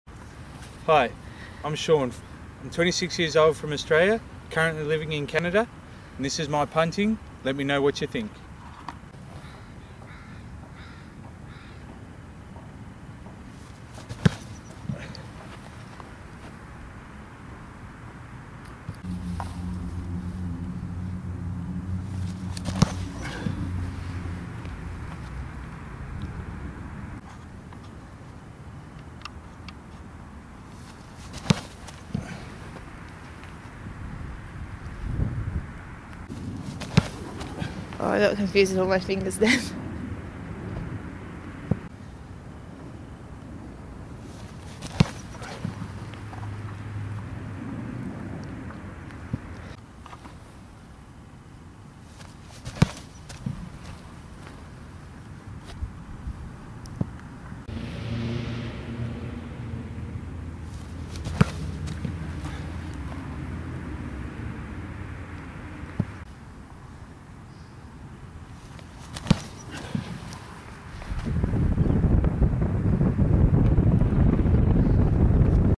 This Aussie can punt!! Punting
Shaun Finlayson, 26 year old Australian, living in Canada. This video shows me punting with both NFL and CFL balls, at distances between 45 and 55 yards from line of scrimmage. My ultimate dream is to play in the NFL. Please take a look and let me know what you think. Constructive criticism is appreciated. Cheers.